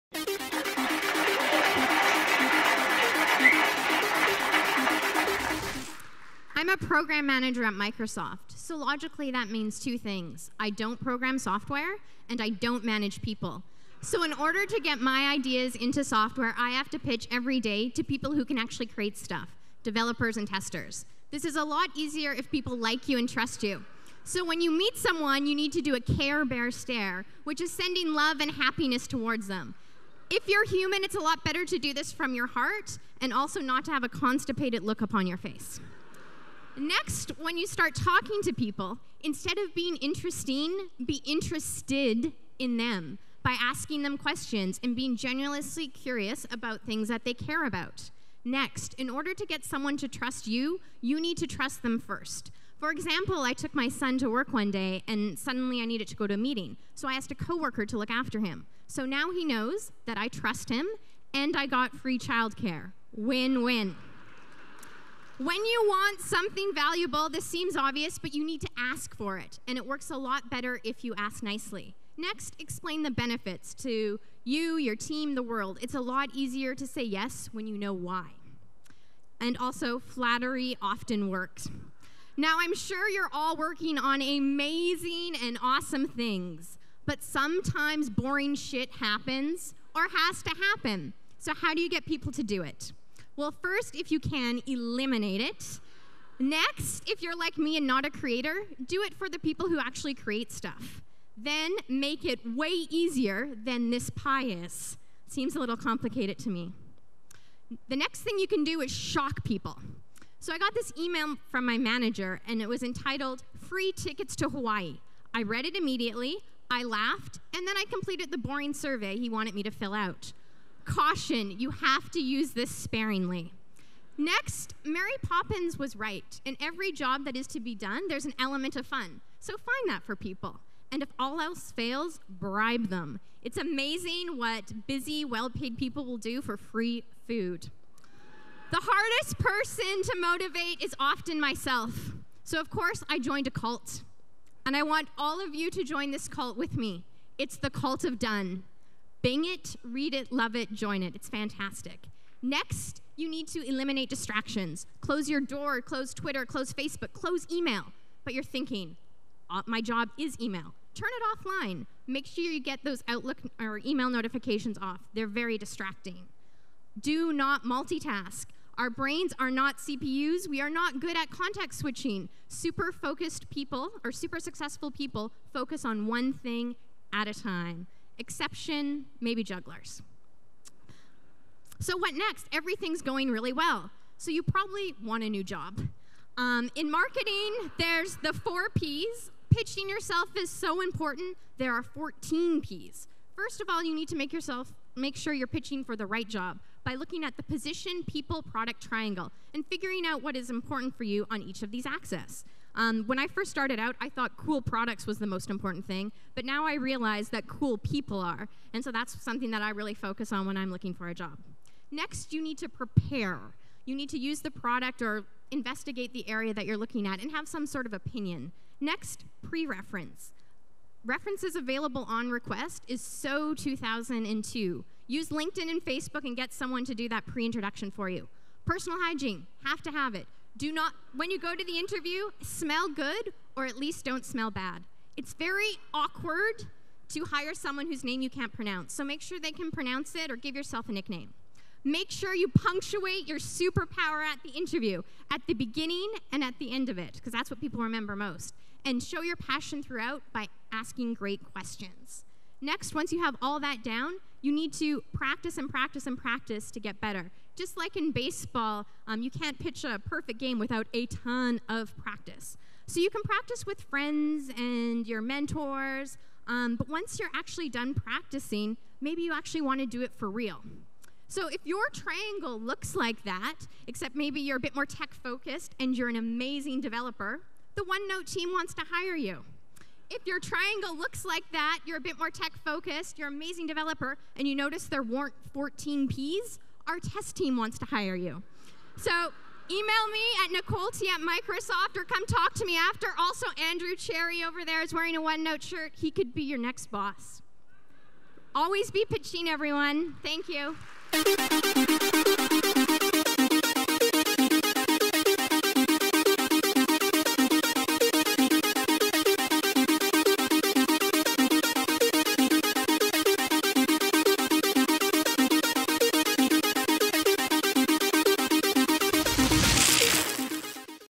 Everyday Pitches - Nicole Steinbok
Learn how to get people to like and trust you, give you time, do boring shit, reply to an email, provide great feedback, buy into plans, hire you and much more!

Nicole Steinbok has been pitching every day at Microsoft for over a decade. She won Microsoft's Presentation Idol for her 22 Minute Meetings pitch. She is most happy with her pitch that enables her to not work on Fridays.